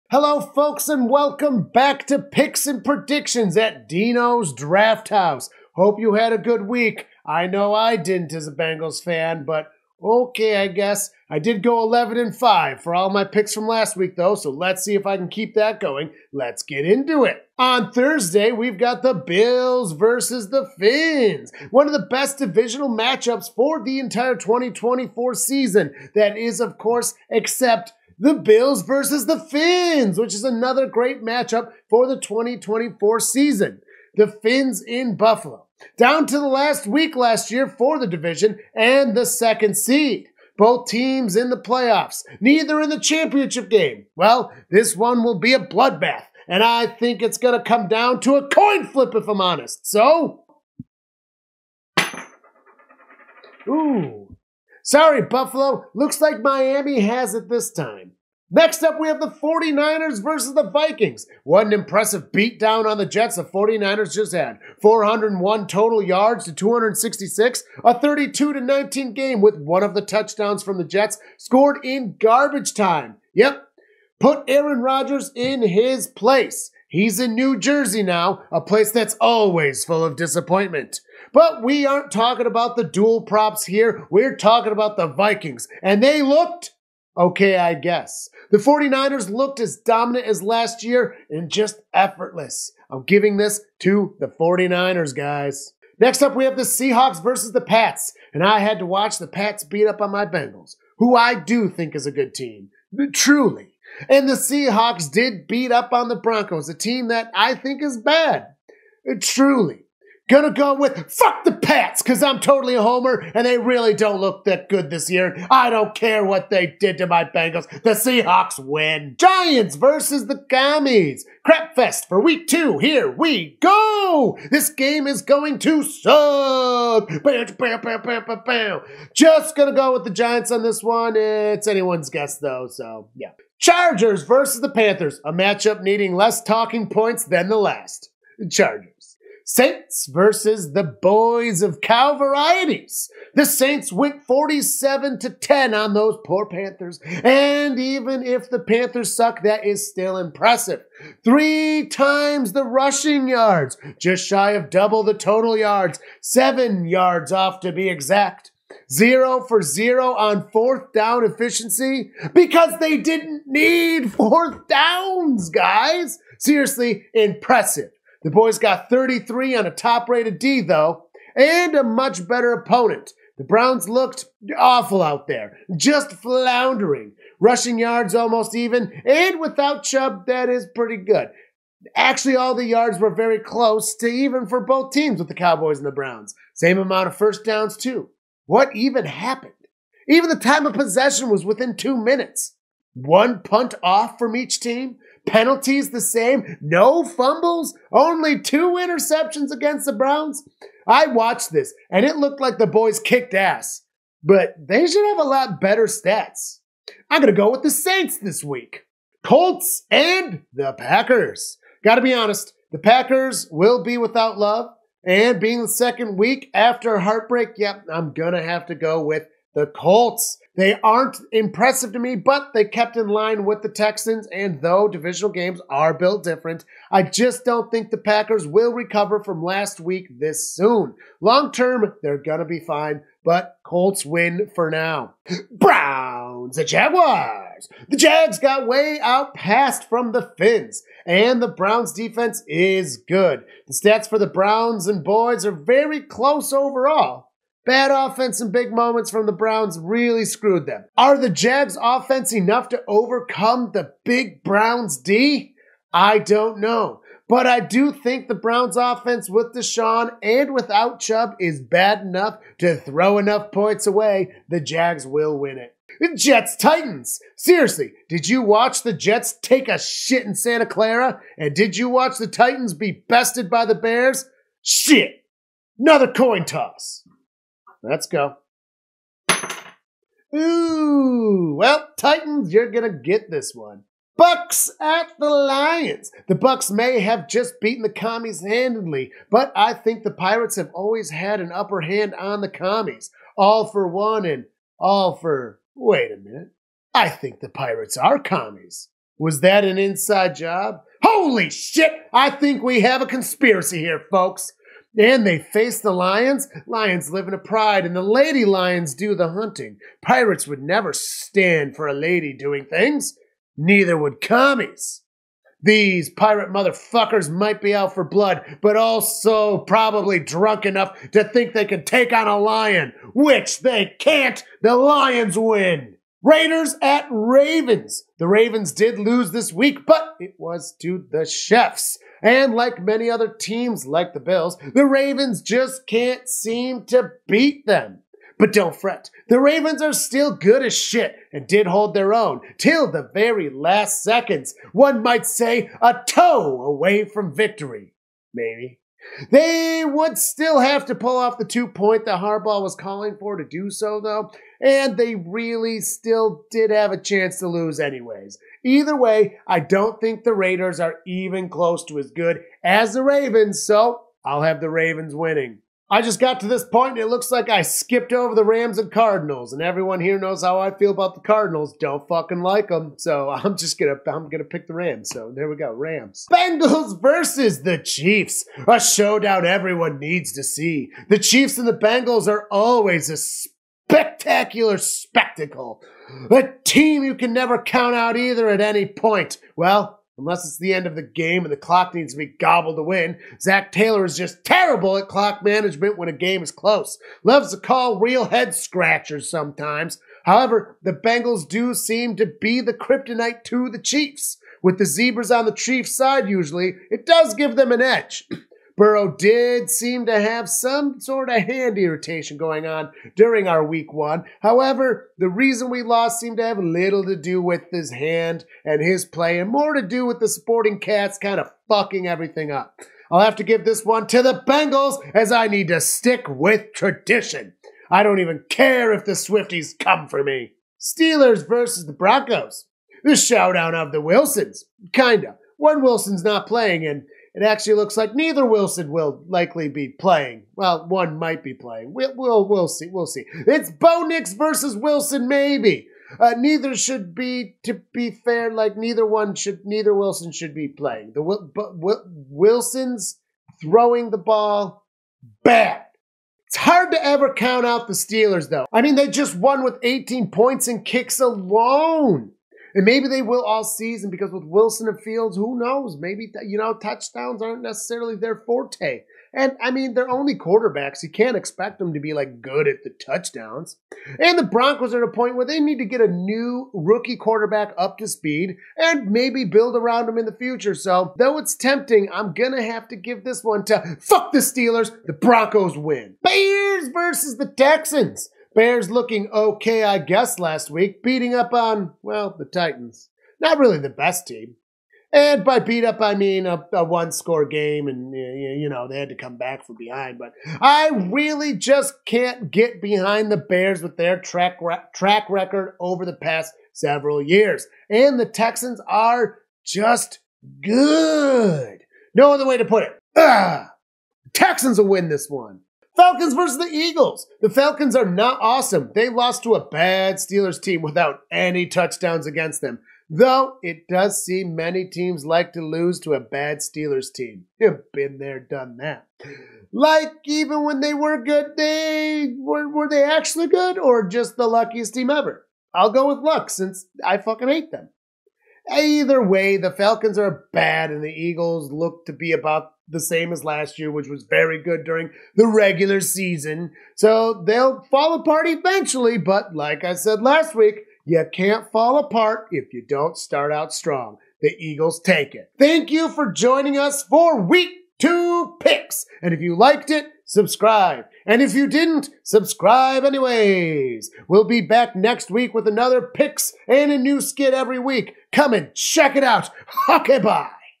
An Idiots Week 2 Picks | NFL 2024
PIcking each NFL Game for week 2 of the 2024 season. I went 12-5 for the first week, and thats with 2 homer picks to boot! lets see if I can keep it going.